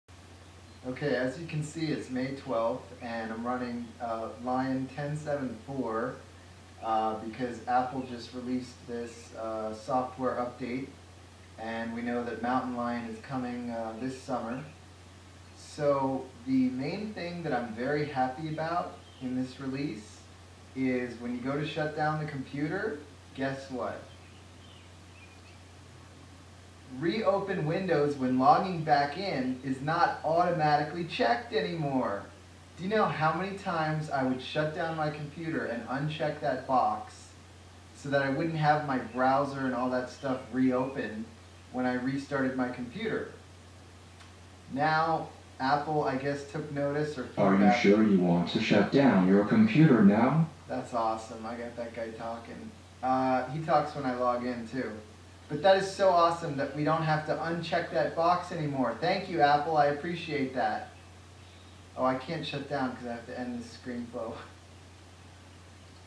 Lion 10.7.4 ReOpen at Login Disabled, Finally
So Glad we don't have to UnCheck the box "Reopen Windows at Login" anymore. Thanks Apple, Seriously. The original feature was probably just to help the Government to see whatever we were recently working on, during the next Home Invasion Raid. But we all know they have a backend to our ISPs, anyways! (Knock, Knock...woops.)

Also, who else is annoyed when Text Edit decides to reopen the last Document you were working on?

Leave your Mac, Windows, and Linux annoyances in the Comments.